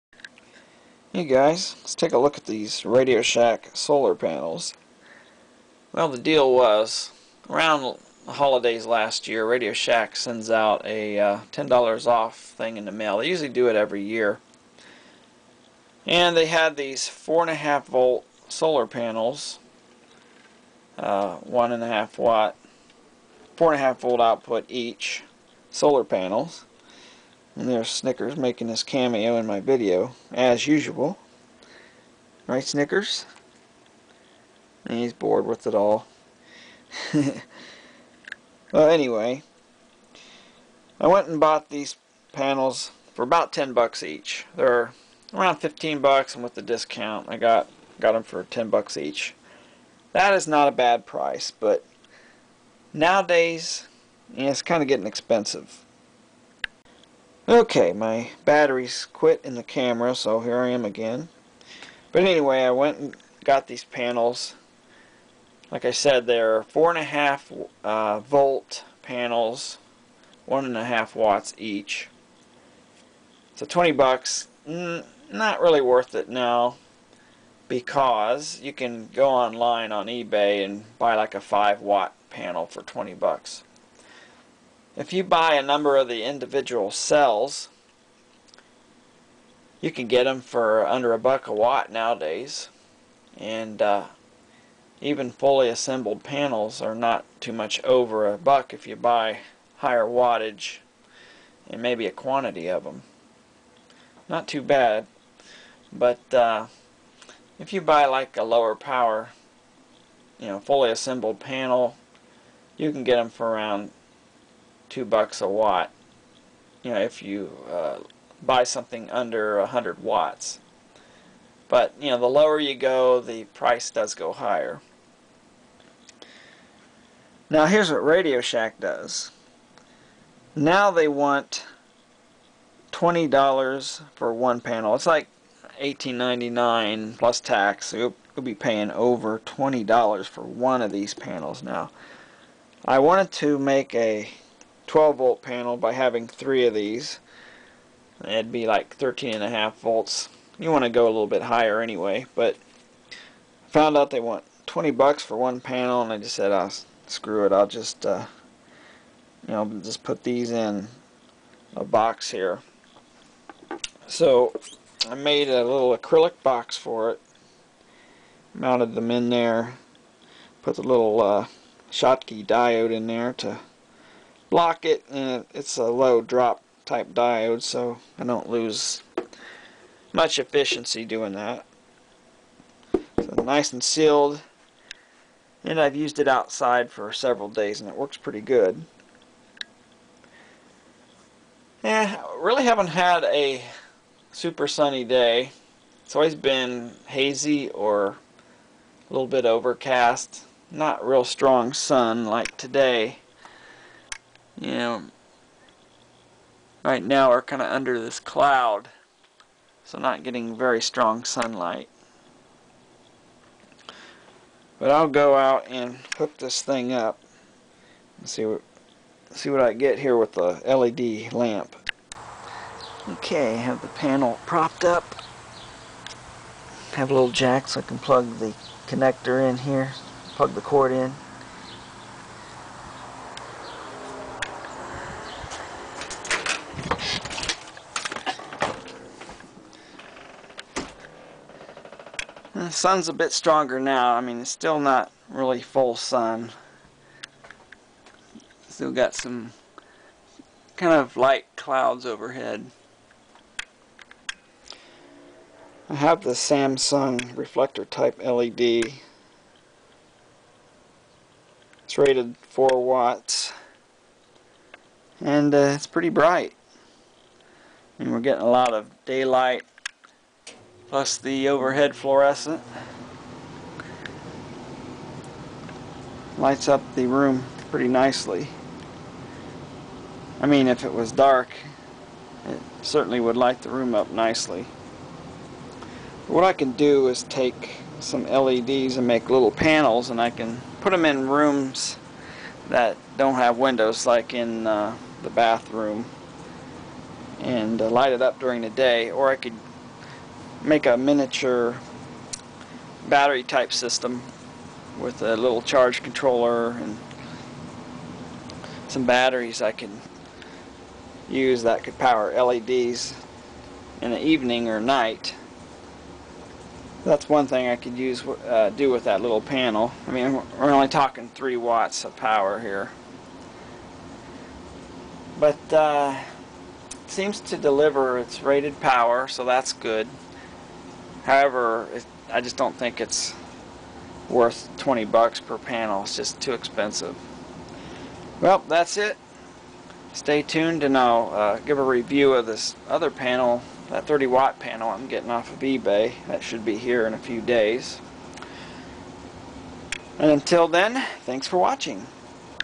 Radio Shack solar panel review
I bought a couple Radio Shack 1.5 watt solar panels to tinker with. See how they work and if they're worth it in this review.